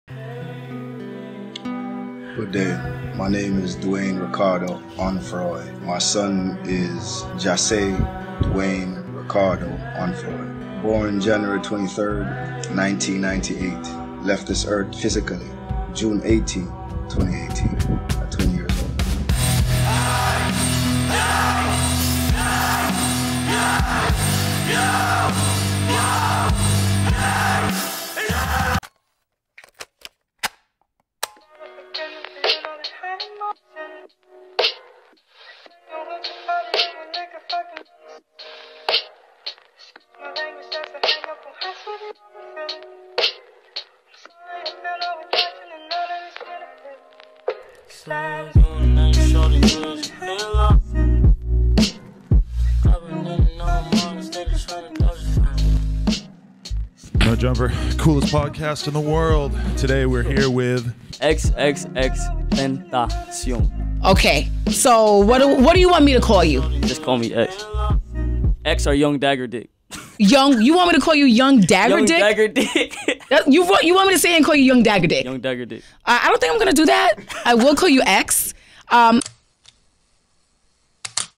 Long Live X: The Story Of XXXTentacion - Trailer (Documentary)
I wasn't really planning on uploading a trailer for this film and this is actually the intro for it, but I might as well reveal what I've been working on for the past two weeks.

The full video should be uploaded in a week or so. It's gonna be at least 30 minutes long, and it's really different from the rest of the content on my channel because I consider this a passion project that I would make on very special occasions.